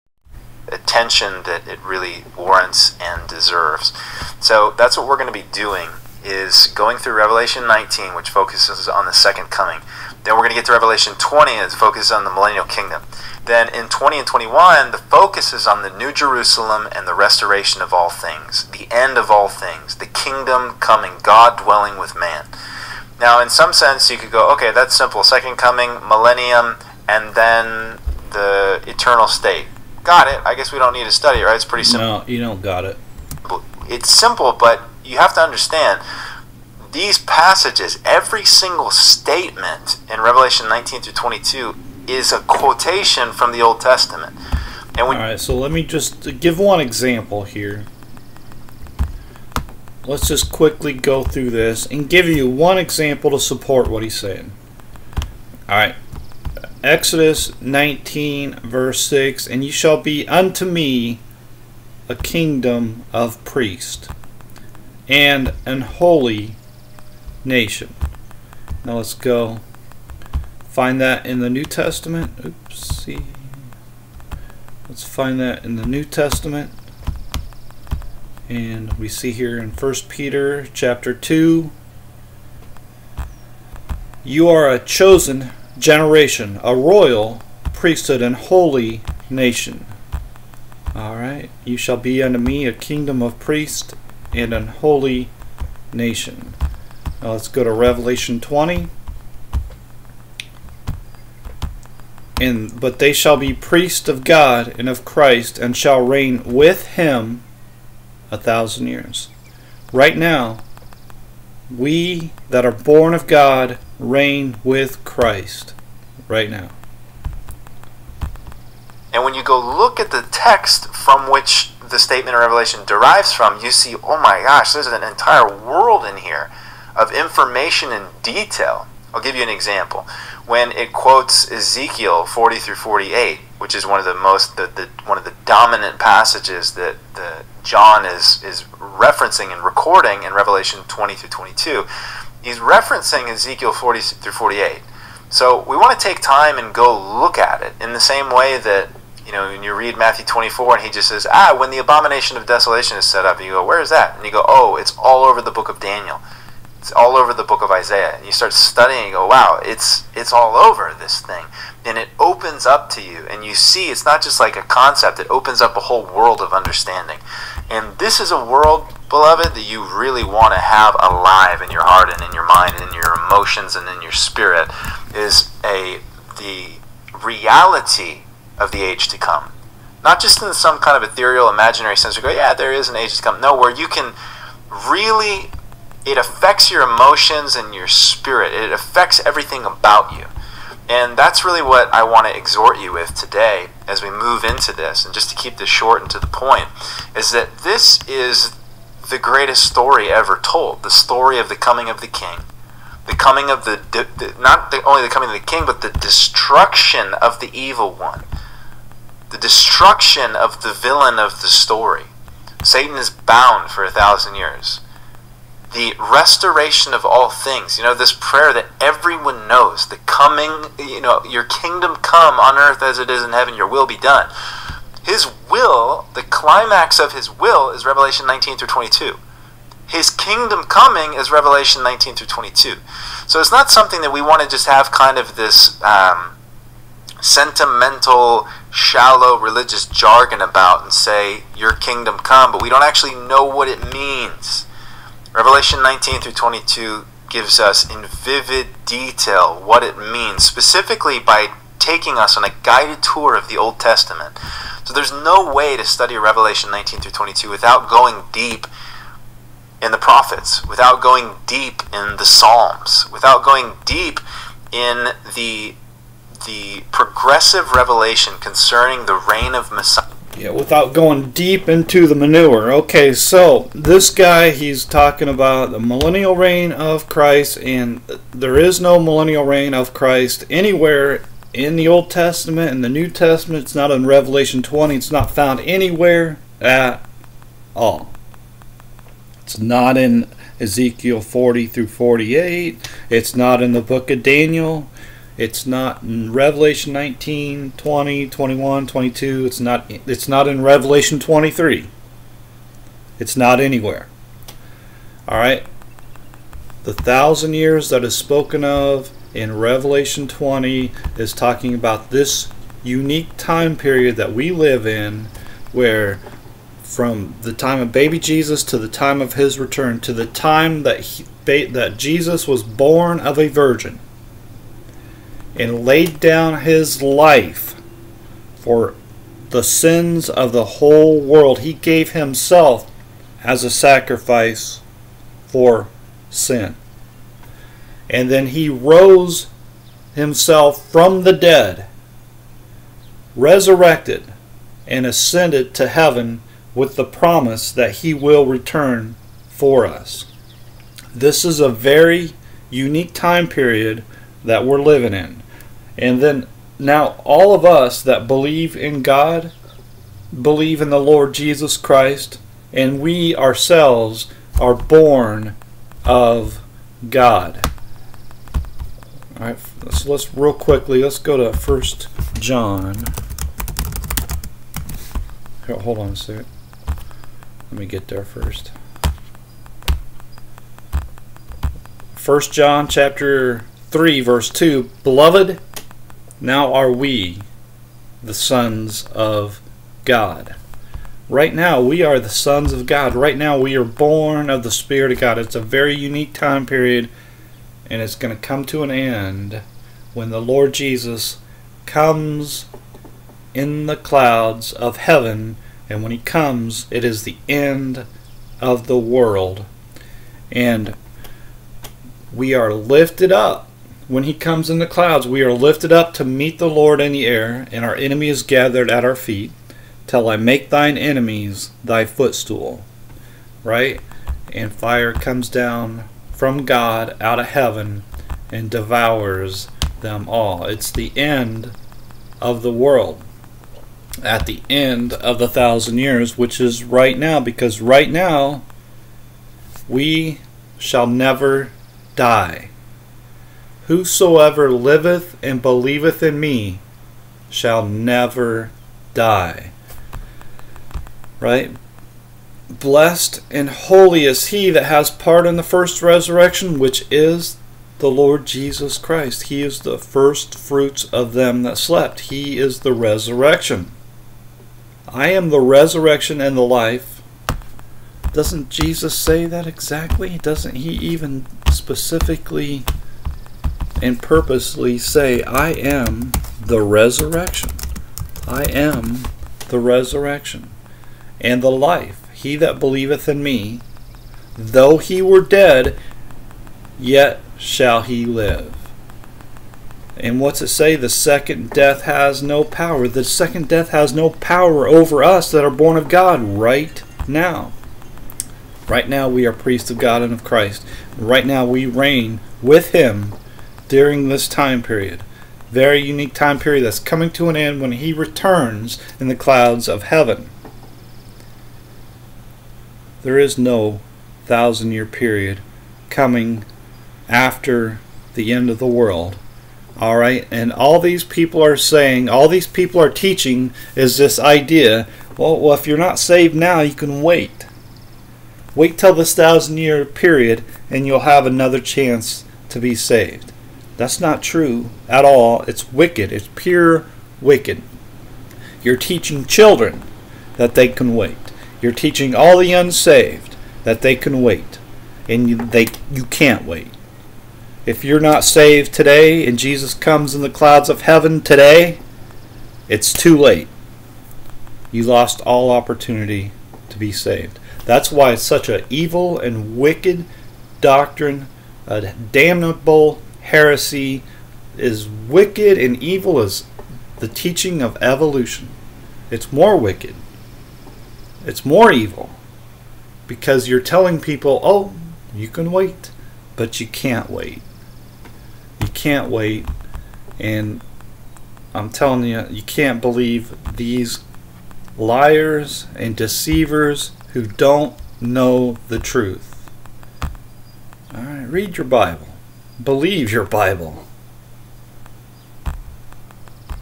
another false teacher teaching a 1000 year reign
there is no 1000 year reign anywhere in the Bible.. at all.